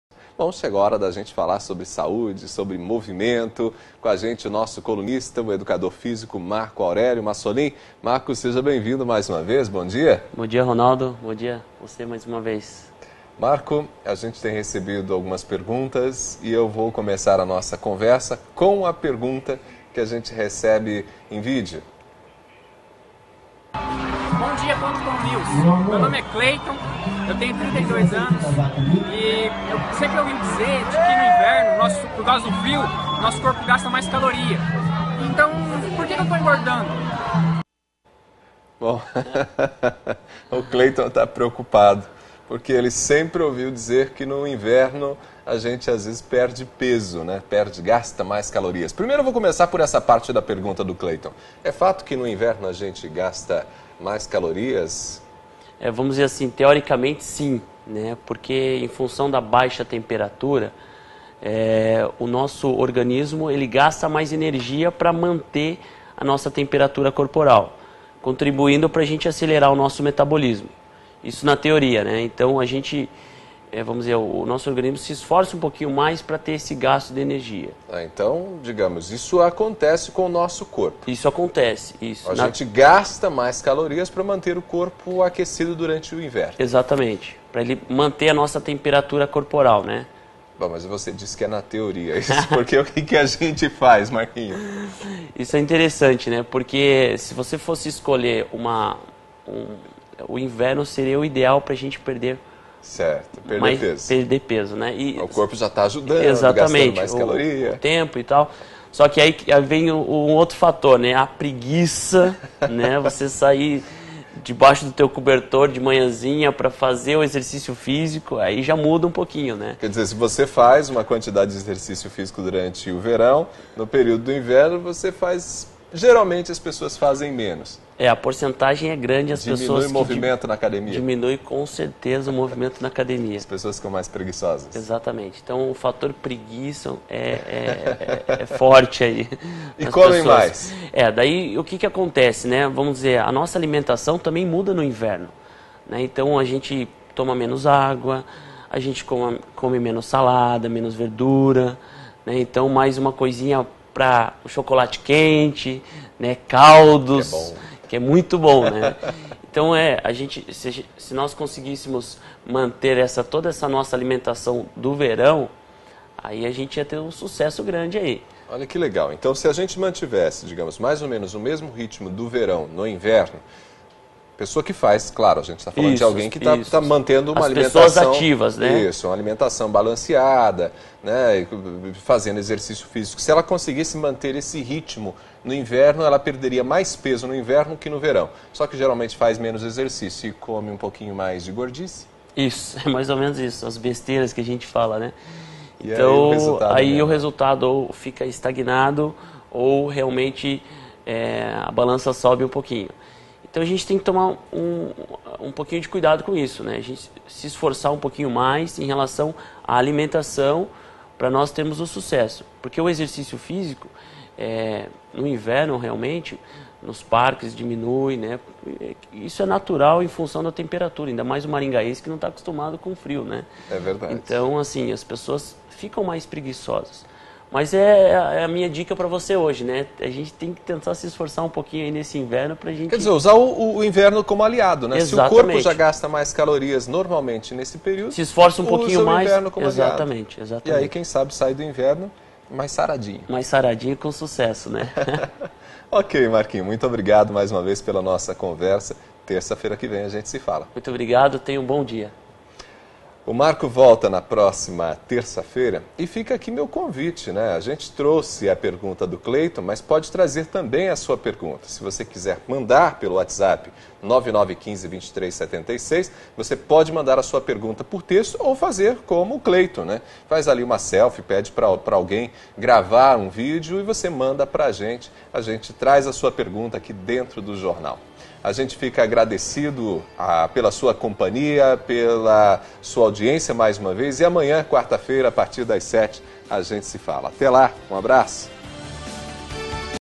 Entrevista com o educador físico Marco Aurélio
Já ouviu dizer que a gente emagrece mais no inverno? Mesmo assim não consegue entender por que você tem engordado com esse friozinho?
O educador físico Marco Aurélio esclarece essas e outras dúvidas na coluna do PontoCom News.